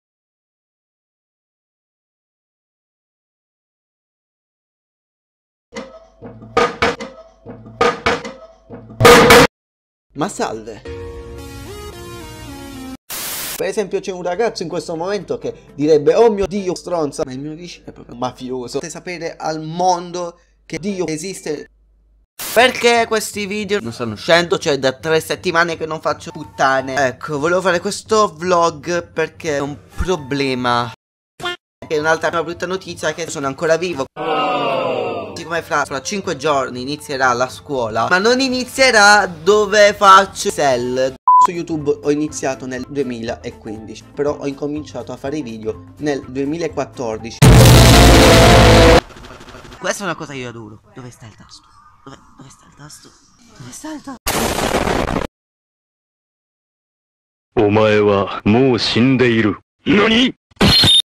YTP GUNWALE
Weilà regazz! Sarò sincero... la ytp non è venuta proprio bene ma avevo veramente pochi vlog a disposizione su cui poter lavorarci su (buona come paraculata?). Comunque a parte gli scherzi spero vi sia piaciuta! Sia a voi che al diretto interessato! Byeee :D